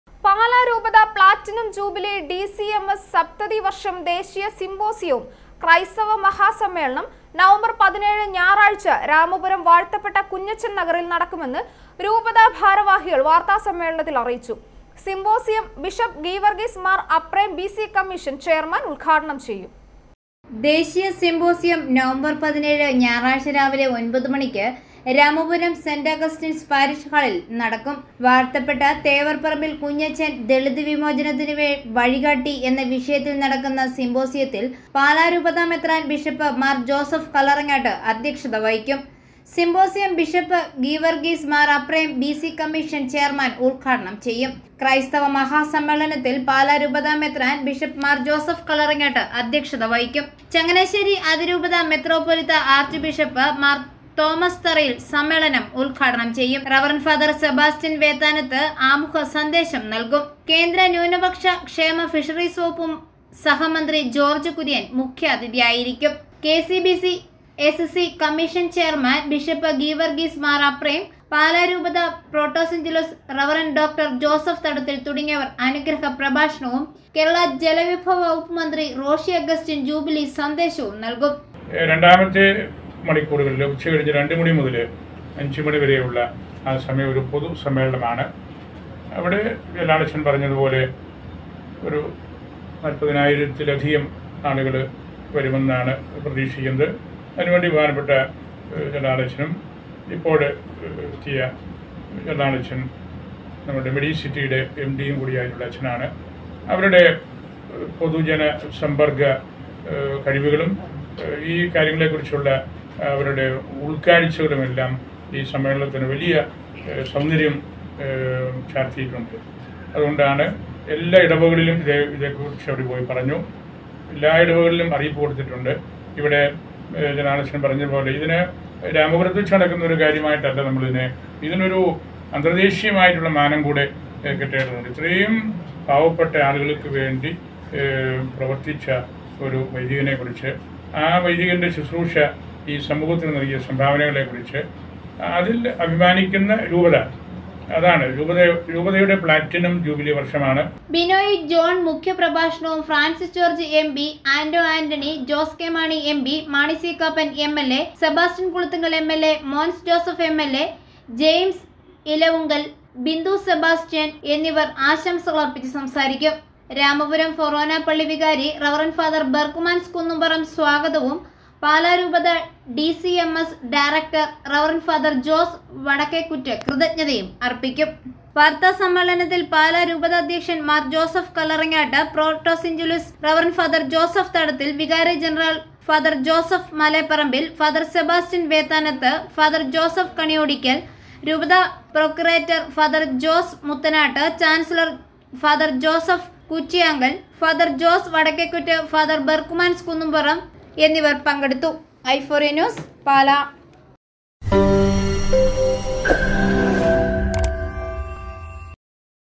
ഡി.സി.എം.എസ് സപ്തതി വര്‍ഷം ദേശീയ സിമ്പോസിയവും ക്രൈസ്തവ മഹാസമ്മേളനംനവംബര്‍ 17 ന്
പാലാ രൂപത പ്ലാറ്റിനം ജൂബിലി  ഡി.സി.എം.എസ് സപ്തതി വര്‍ഷം  ദേശീയ സിമ്പോസിയവും ക്രൈസ്തവ മഹാസമ്മേളനം
നവംബര്‍ 17 ഞായറാഴ്ച രാമപുരം വാഴ്ത്തപ്പെട്ട കുഞ്ഞച്ചന്‍ നഗറില്‍ നടക്കുമെന്ന് രൂപത ഭാരവാഹികള്‍ വാര്‍ത്ത സമ്മേളനത്തില്‍ അറിയിച്ചു.സിമ്പോസിയം ബിഷപ്പ് ഗീവര്‍ഗീസ് മാര്‍ അപ്രേം ബി.സി കമ്മീഷന്‍ ചെയര്‍മാന്‍ ഉദ്ഘാടനം ചെയ്യും